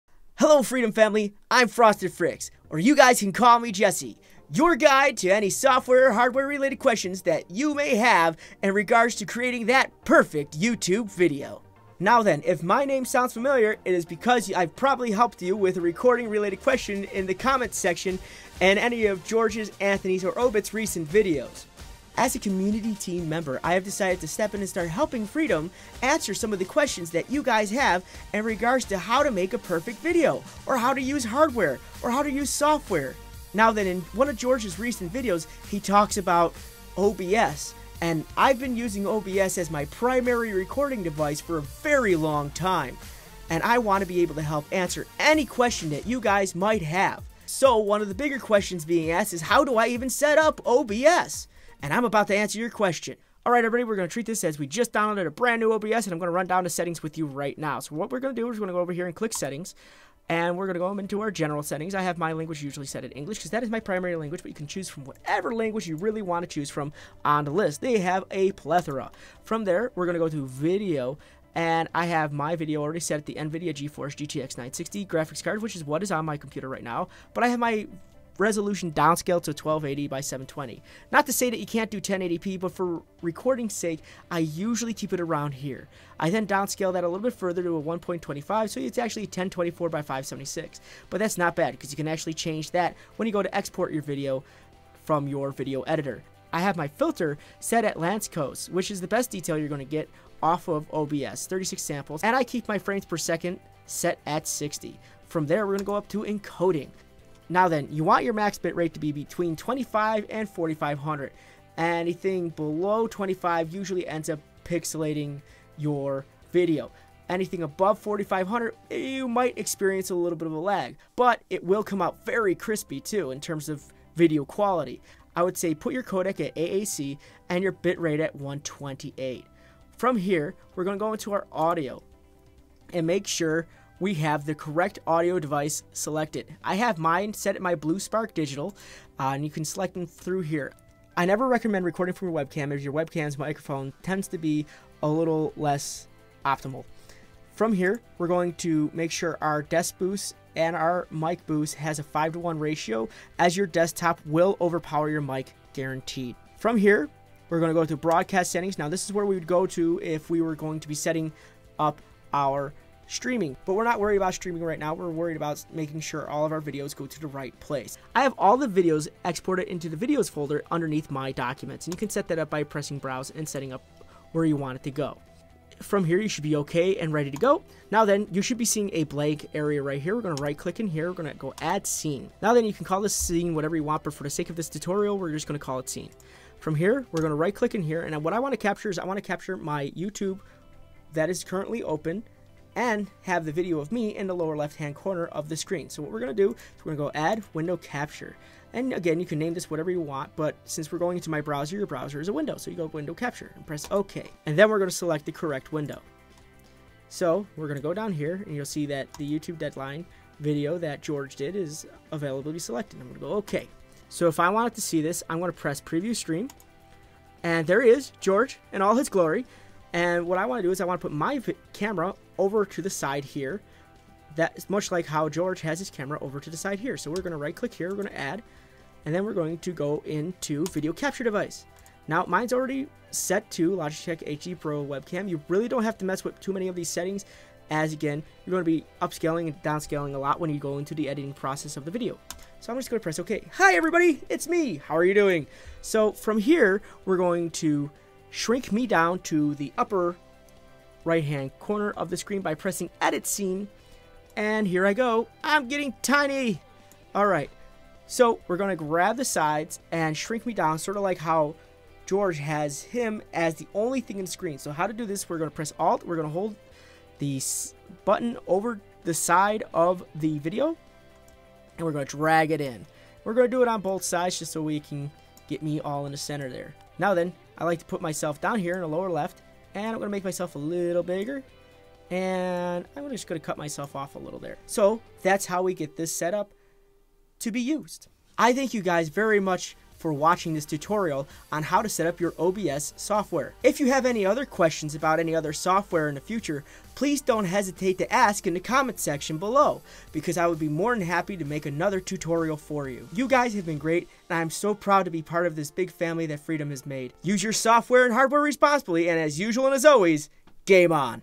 OBS Set Up Tutorial Pilot For Freedom Network
In this episode we learn about How to set up our OBS recording software!

This is a pilot episode I did for my network that i belong to called Freedom network. They are an awesome family to be a part of and I am glad to have at least tried this out! I tried to make this short and sweet. There are many others out there like this one, but they are well over 10 to 15 minutes and explain the same information! in the end I hope I am considered for the responsibility!